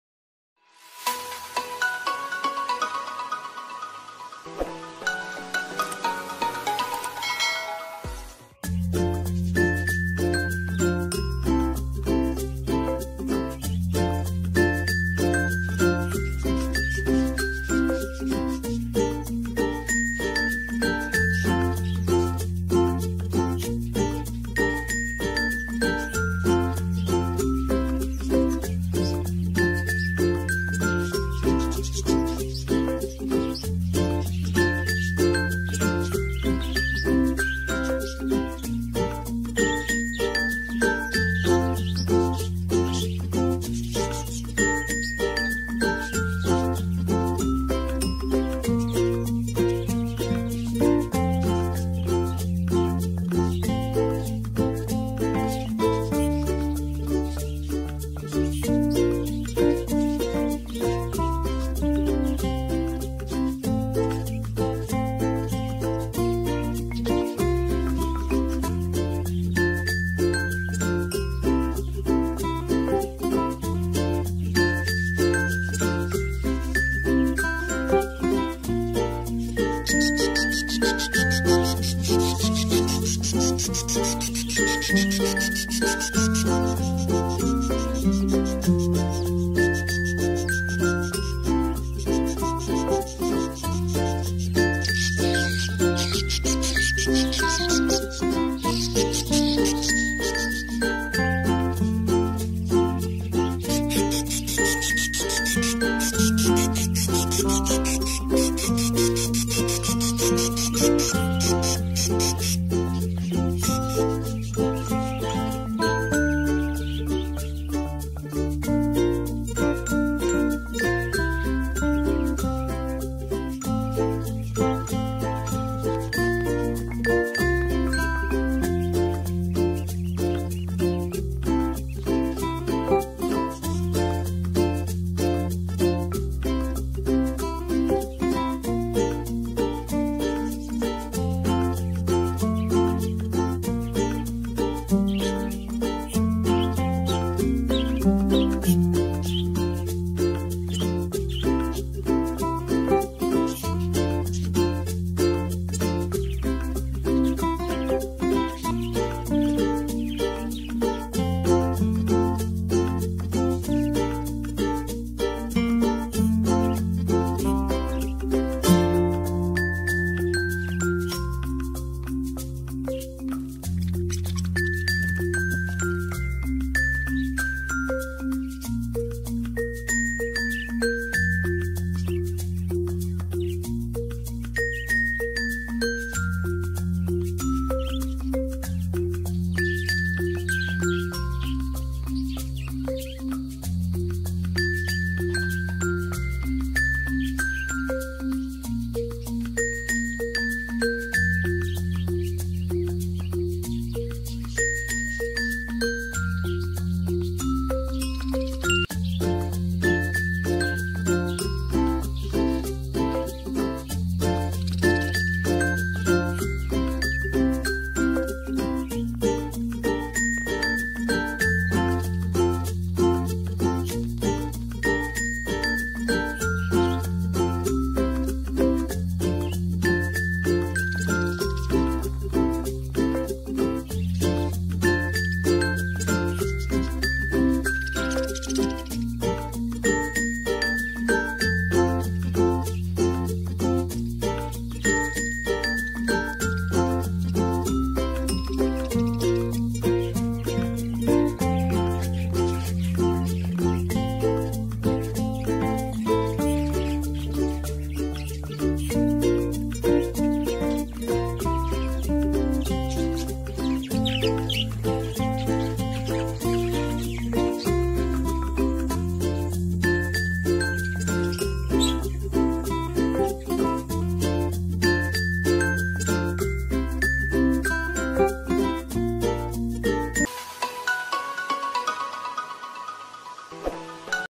BIRDS UPDATE: DUMAMI NANAMAN SILA
Hello, guys! Medyo hindi tayo masyadong active noong nakaraang linggo dahil sa busy schedule. Ngunit ngayon ako'y nagbabalik up ang magbigay ng update sa mga inaalagaan kong mga ibon. Dumami nanaman sila! Paano nga ba? Ang sikretong malupit ay nabunyag na!

Watch these videos para malaman ang sikreto!

Salamat sa suporta!